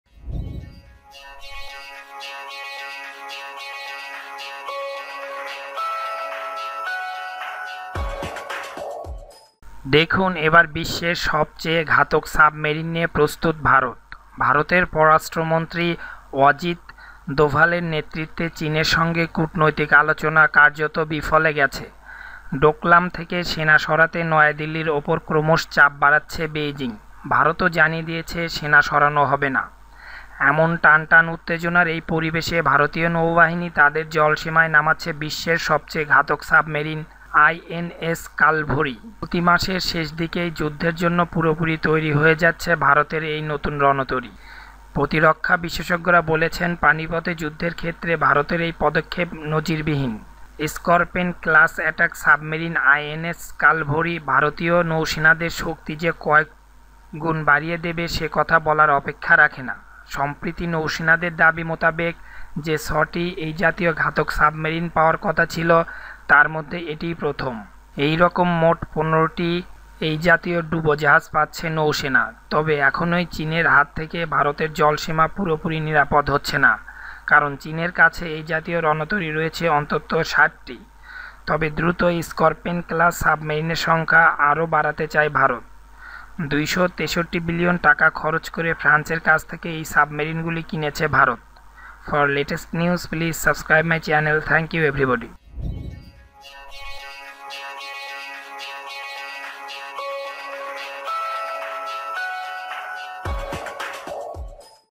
দেখুন এবার বিশ্বের সবচেয়ে ঘাতক সাবমেরিন নিয়ে প্রস্তুত ভারত
ভারতের পররাষ্ট্র মন্ত্রী অজিত দোভালের নেতৃত্বে চীনের সঙ্গে কূটনৈতিক আলোচনা কার্যত বিফলে গেছে। ডোকলাম থেকে সেনা সরাতে নয়াদিল্লির ওপর ক্রমশ চাপ বাড়াচ্ছে বেইজিং। ভারতও জানিয়ে দিয়েছে, সেনা সরানো হবে না।

এমন টানটান উত্তেজনার এই পরিবেশে ভারতীয় নৌবাহিনী তাদের জলসীমায় নামাচ্ছে বিশ্বের সবচেয়ে ঘাতক সাবমেরিন ‘আইএনএস কালভরি’। চলতি মাসের শেষদিকেই যুদ্ধের জন্য পুরোপুরি তৈরি হয়ে যাচ্ছে ভারতের এই নতুন রণতরী।

প্রতিরক্ষা বিশেষজ্ঞরা বলছেন, পানিপথে যুদ্ধের ক্ষেত্রে ভারতের এই পদক্ষেপ নজিরবিহীন। স্করপেন ক্লাস অ্যাটাক সাবমেরিন আইএনএস কালভরি ভারতীয় নৌসেনার শক্তি যে কয়েকগুণ বাড়িয়ে দেবে, সে কথা বলার অপেক্ষা রাখে না।

সম্প্রতি নৌসেনার দাবি মোতাবেক যে ছ’টি এই জাতীয় ঘাতক সাববেরিন পাওয়ার কথা ছিল, তার মধ্যে এটিই প্রথম। এরকম মোট ১৫টি এই জাতীয় ডুবোজাহাজ পাচ্ছে নৌসেনা।

তবে এখনই চীনের হাত থেকে ভারতের জলসীমা পুরোপুরি নিরাপদ হচ্ছে না। কারণ চীনের কাছে এই জাতীয় রণতরী রয়েছে অন্তত ৬০টি। তবে দ্রুতই স্করপেন ক্লাস সাবমেরিনের সংখ্যা আরও বাড়াতে চায় ভারত। ২৩৬ বিলিয়ন টাকা খরচ করে ফ্রান্সের কাছ থেকে এই সাবমেরিনগুলি কিনেছে ভারত।